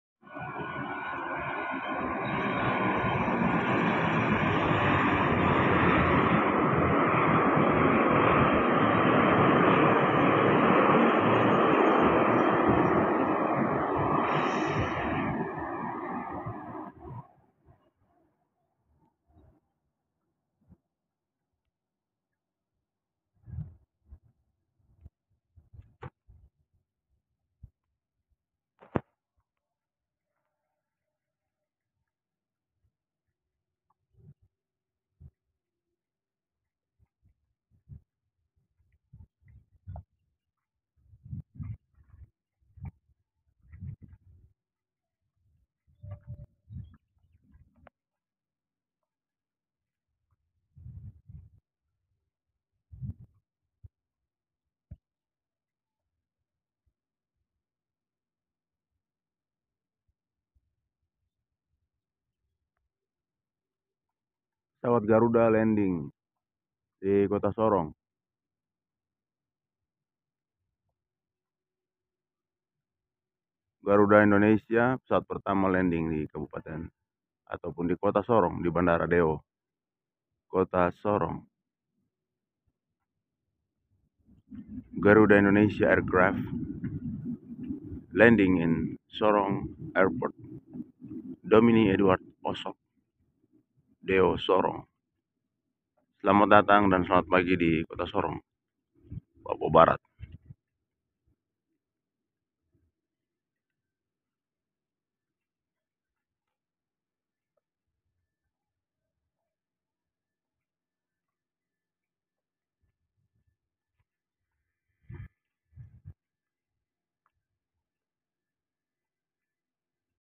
Pesawat Garuda 2 Menit Before Landing Sorong Airport
Pulau Doom View, Pesawat Garuda 2 Menit Before Landing Sorong Airport, Pesawat Pertama Pagi Hari Landing Di Sorong